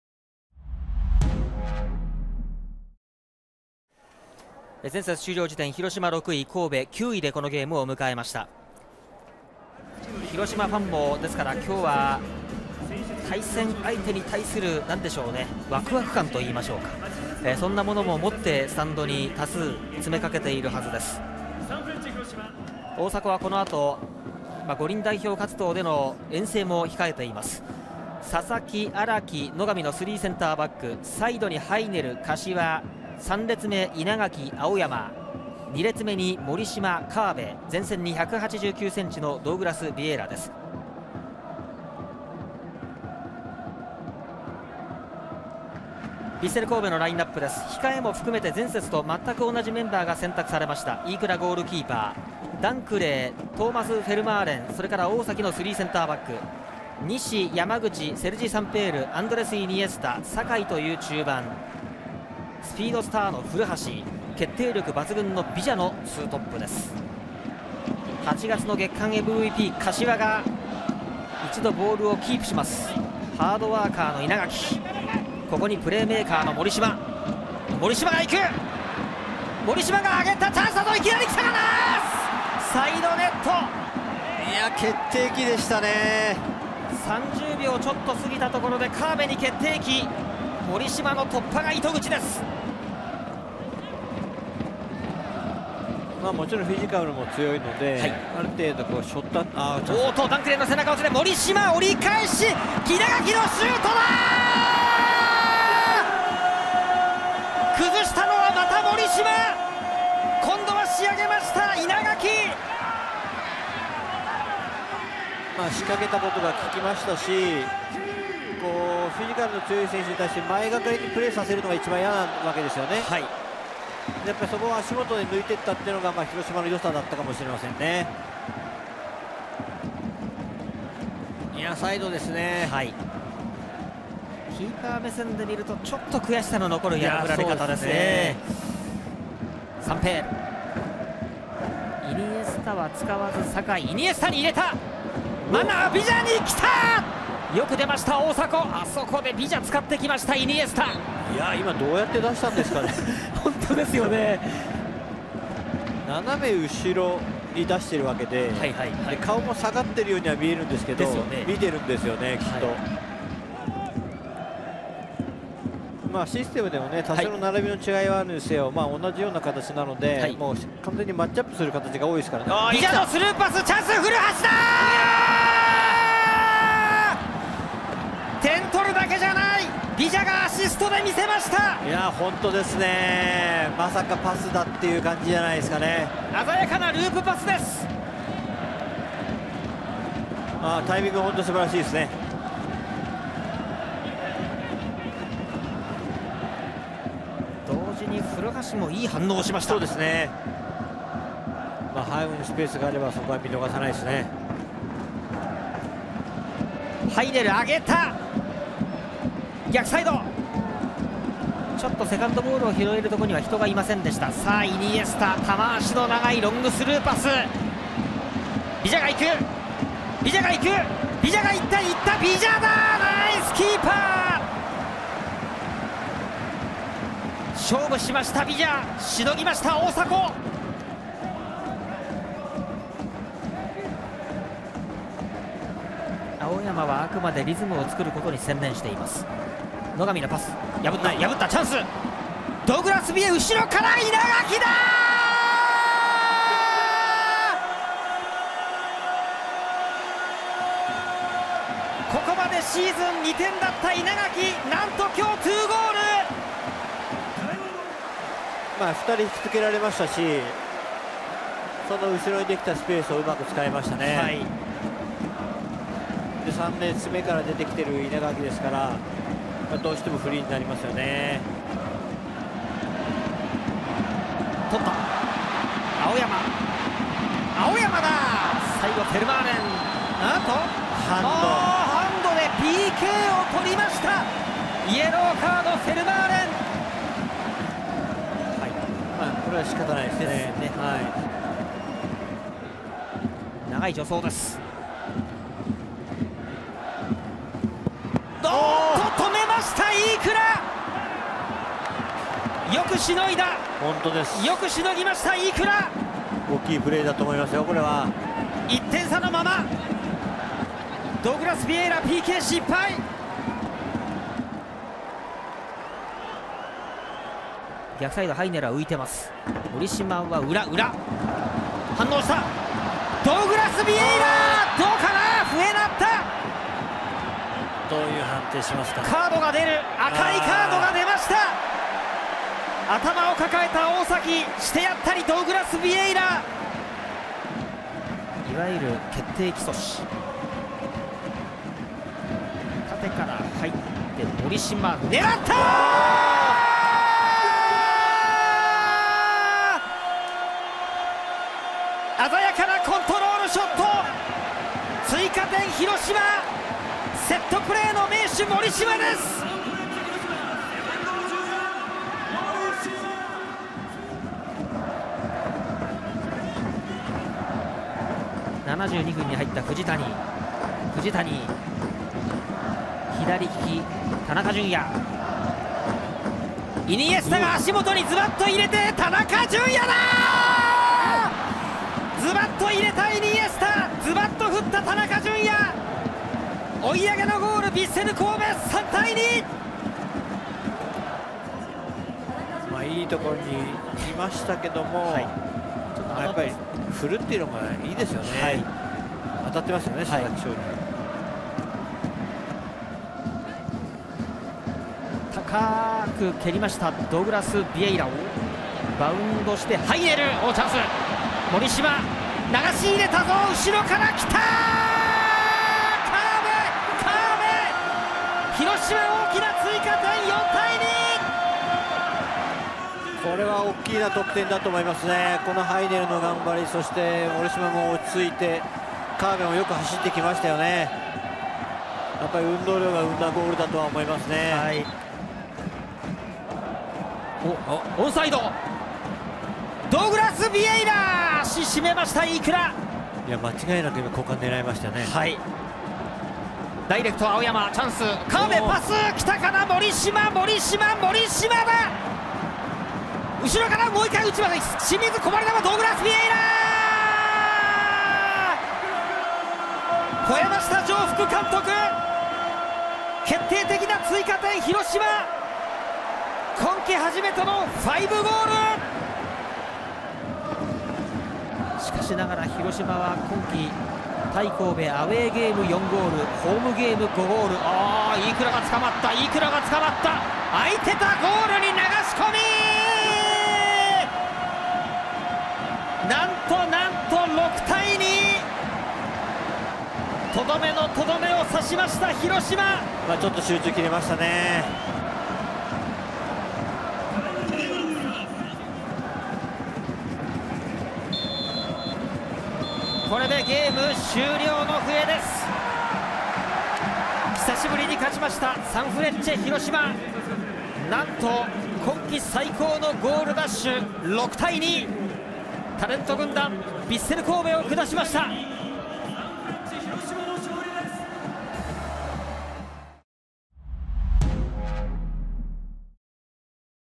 明治安田生命J1リーグ第28節vs.ヴィッセル神戸 試合ハイライト
⚽明治安田生命J1リーグ第28節　試合結果⚽
サンフレッチェ広島　6-2　ヴィッセル神戸
前半：2-1
後半：4-1
【得点者（広島のみ）】稲垣祥選手（2）、森島司選手（2）、川辺駿選手、ドウグラス・ヴィエイラ選手

ホームでの歴代10位となる29,666名の皆さまとともに6得点を奪い、快勝！！
たくさんの熱いご声援をありがとうございました！